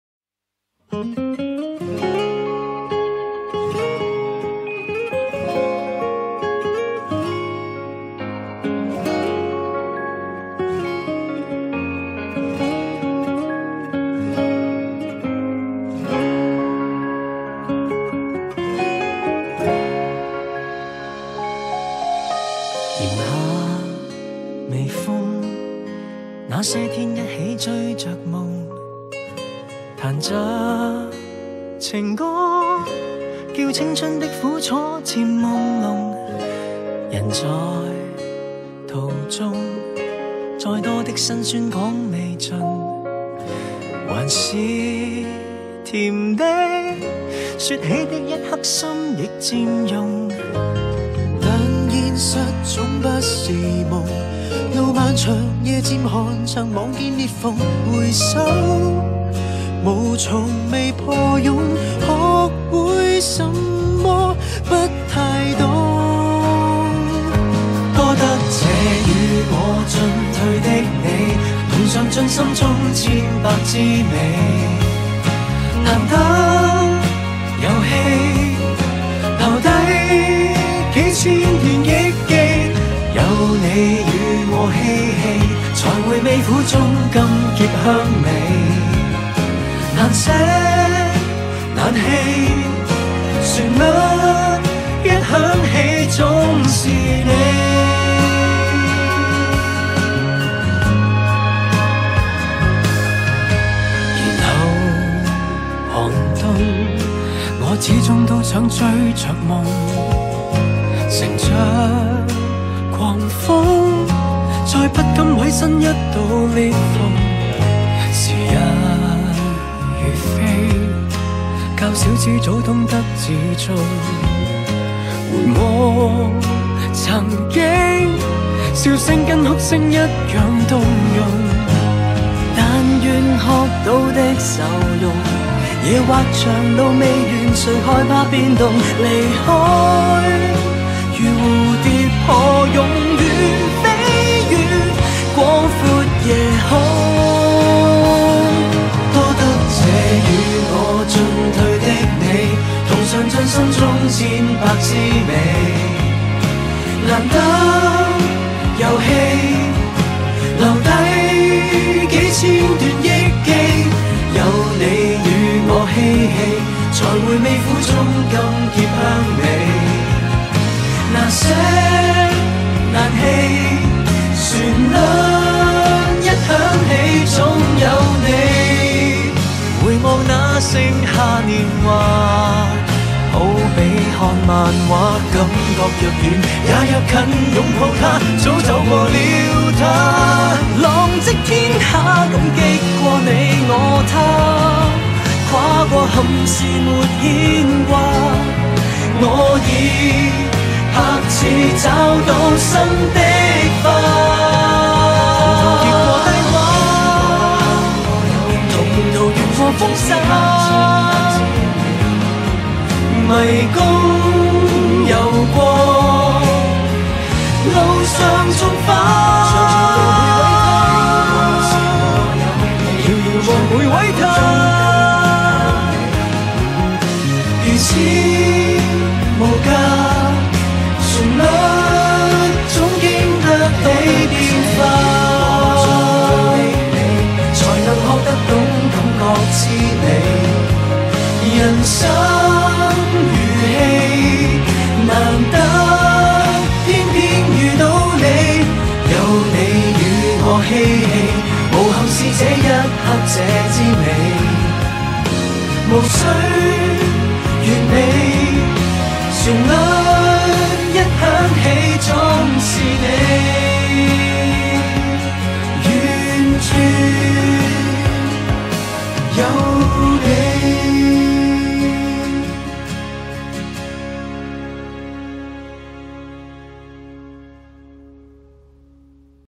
伍德基校長榮休祝福片段
本校伍德基校長榮休
師生祝福片段